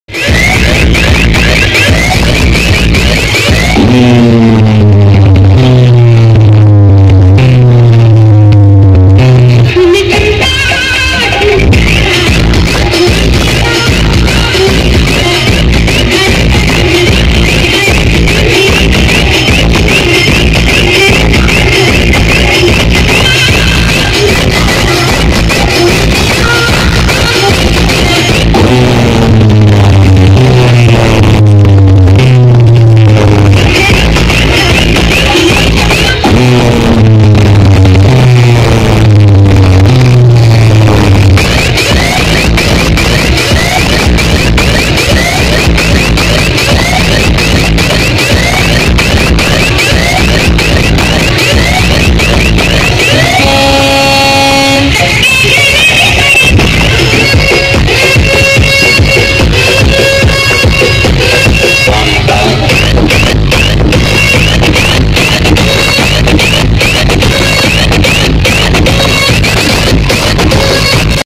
Dj competition Chintamani Vs jay ganesh Vs prathmesh sound pune shiv jayanti
Dj competition  Chintamani Vs jay ganesh Vs prathmesh sound  pune shiv jayanti kalas danori